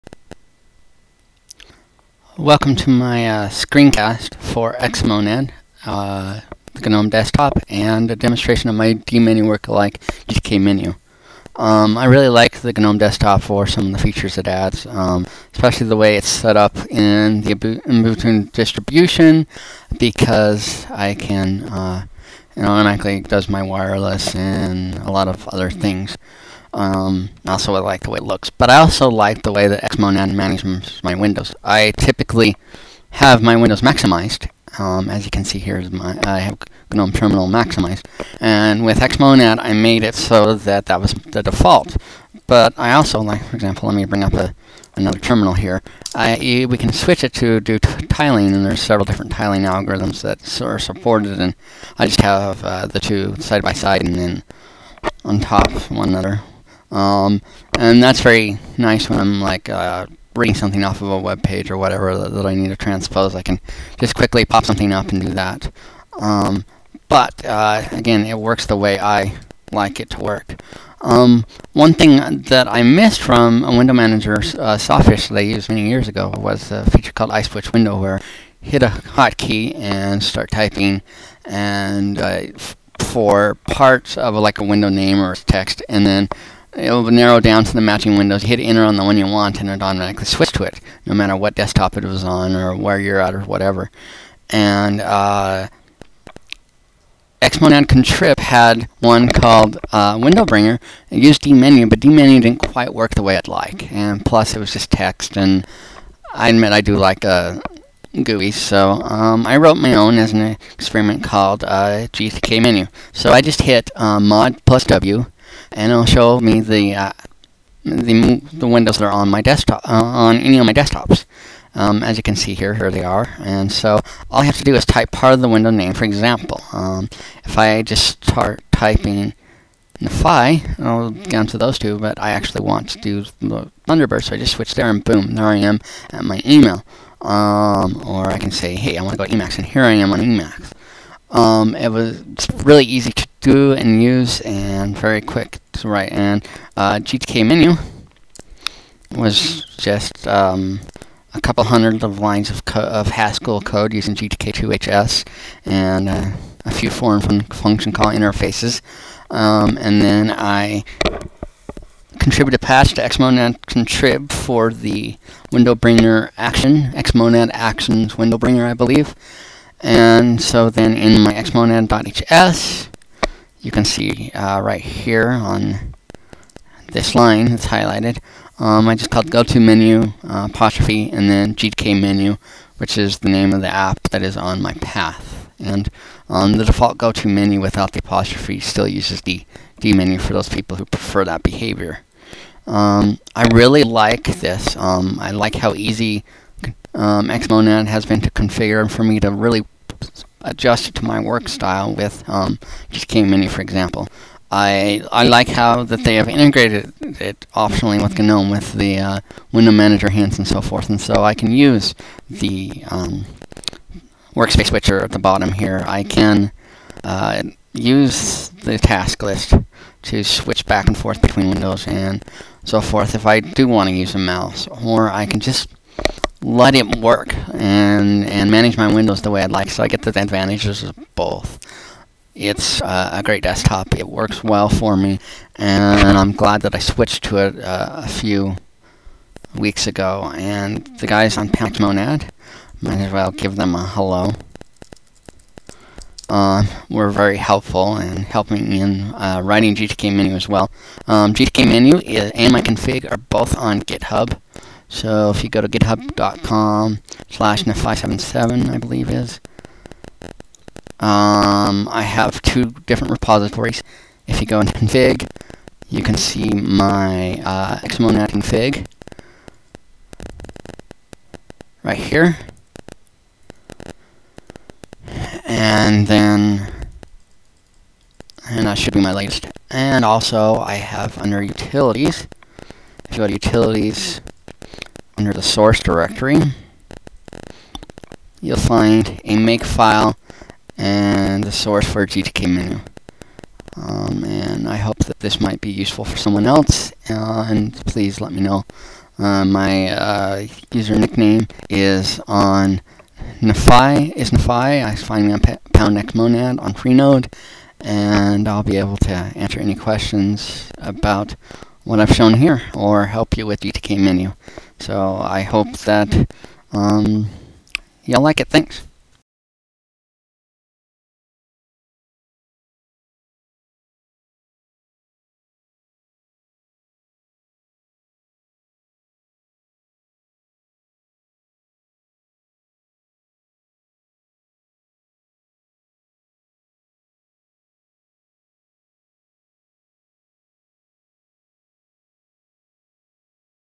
XMonad, Gnome, and gtkmenu screencast
A quick screencast demonstrating XMonad, Gnome, and gtkmenu